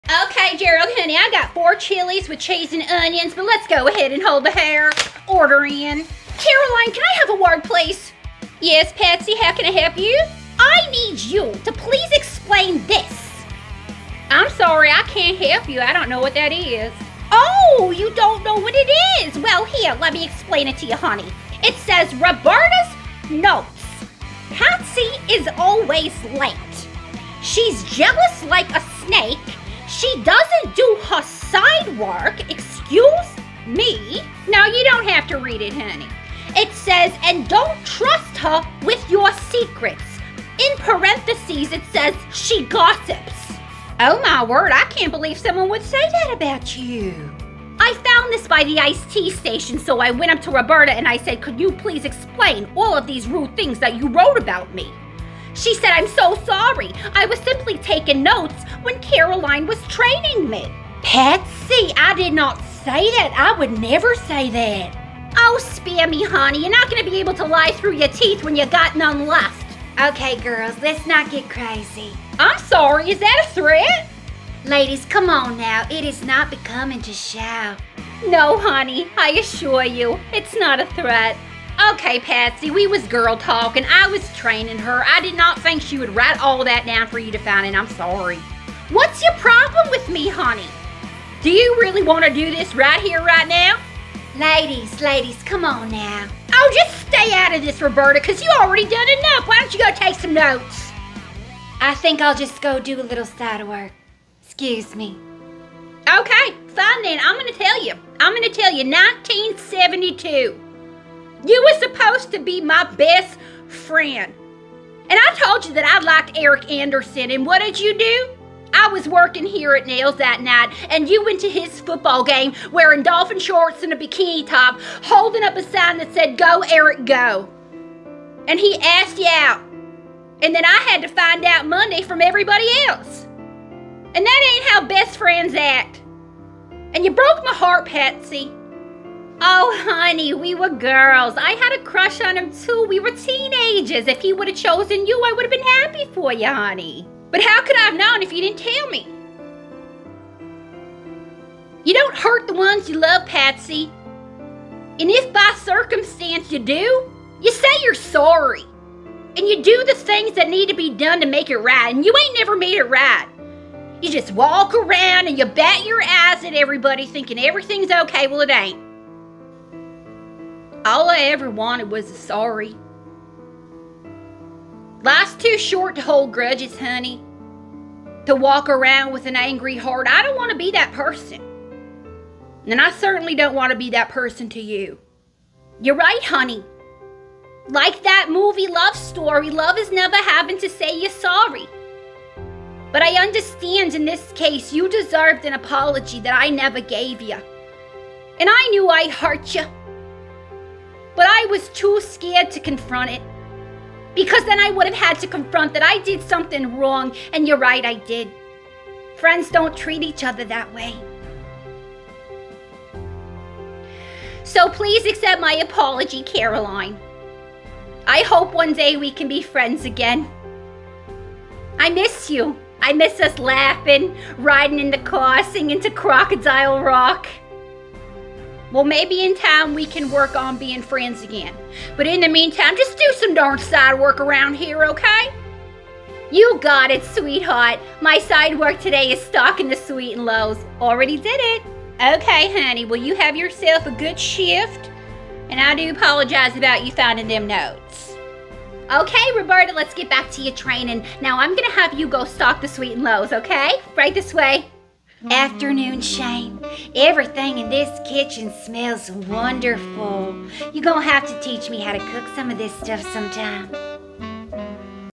Nell's Diner, 1985!
Nell's Diner, 1985! The only thing hotter than the chili is the drama! The tension between Caroline & Patsy hits its boiling point, and those who can't take the heat better get out the kitchen, hunnie! 🔥

Written, Acted & Directed by Casey Dressler